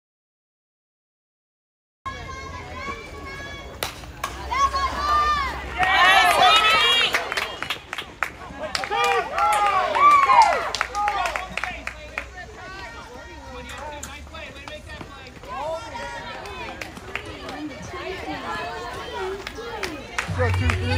Lanie catches fly Out vs Shooting Stars 2030 Dolby
#99 Lanie Catches Fly Out
Vs Shooting Stars 2030 (Dolby)
Ashburn Shooting Stars 14U Osgood
Coach Dan Osgood
Fall 2024 Season
August 24 Games
Trailside Park - Ashburn, VA

HEAD COACH:
  Dan Osgood

ASSISTANT COACHES:
  Karl Schwanda
  Tommy Bell